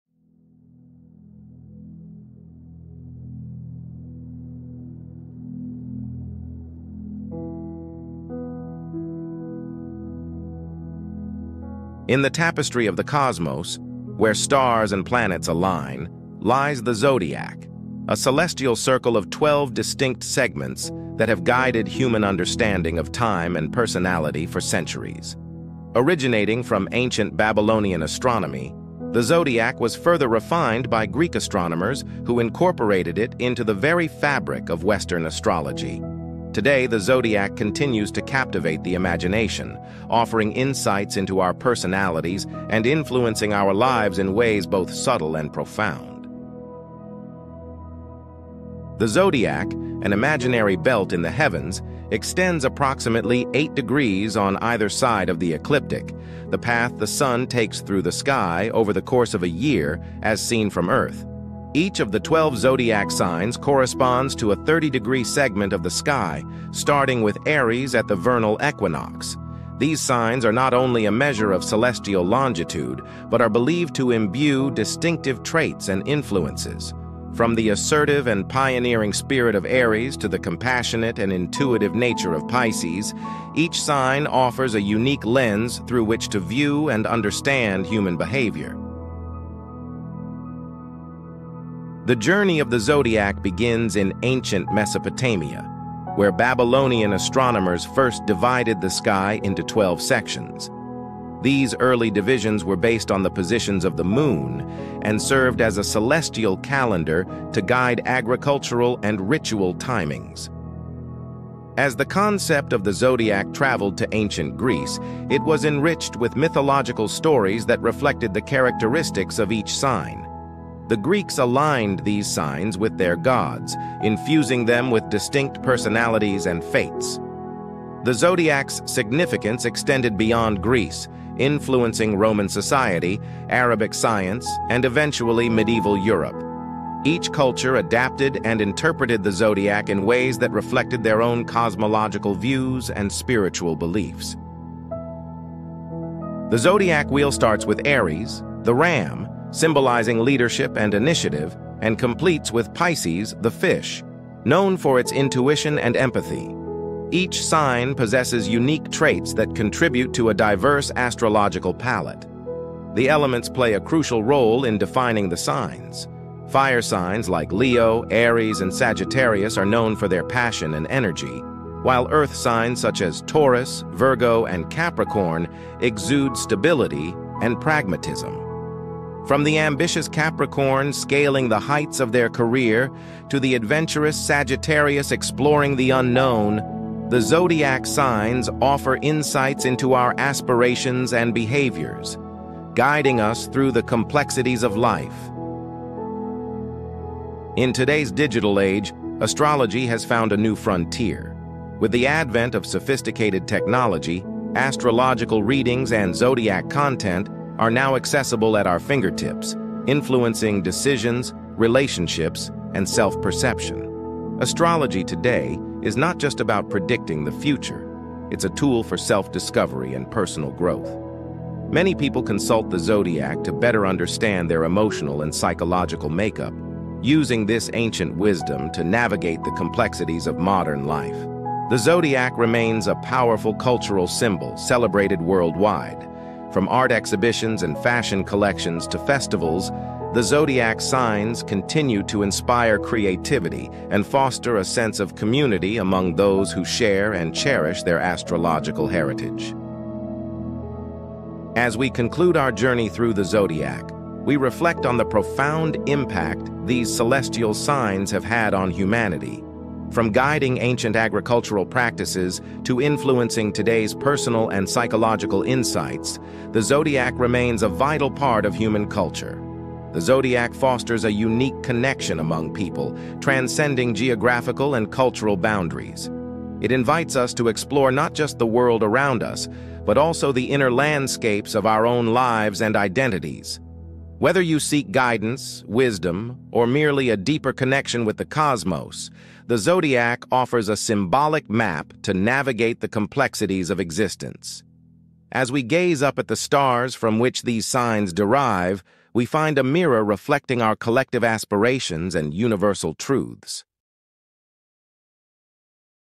✨ Understanding the Zodiac: A Celestial Guide 🌌
Dive into the mysteries of the zodiac in this enlightening episode! 🌠

Discover how these celestial signs have guided cultures across the ages and continue to influence our lives today. 🌍🔮